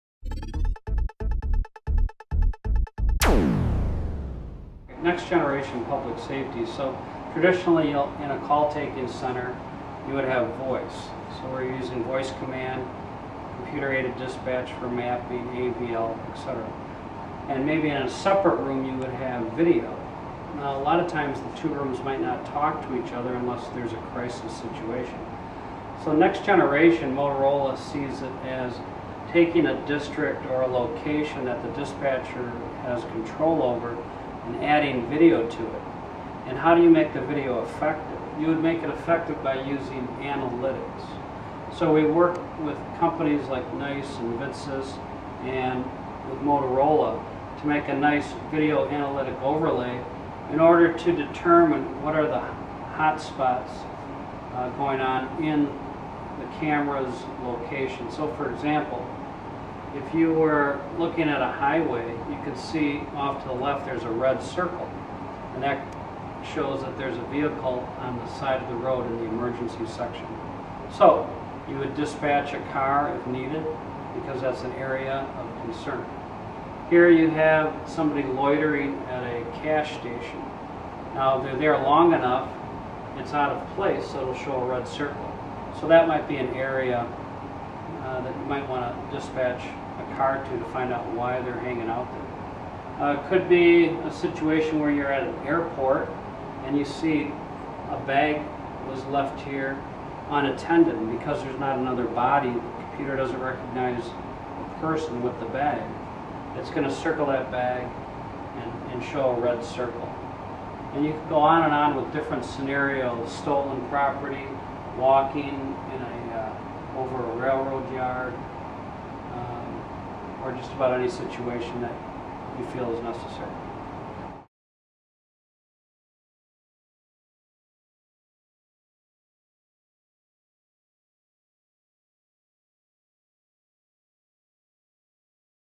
Next Generation Public Safety Technology at Motorola Solutions
We're back at Motorola Solution's Innovation Center to bring you another inside look at new technology. Our expert Wayne Weisser shows us how call in-take centers can combine both voice and video with Next Generation Public Safety technology in this live demo.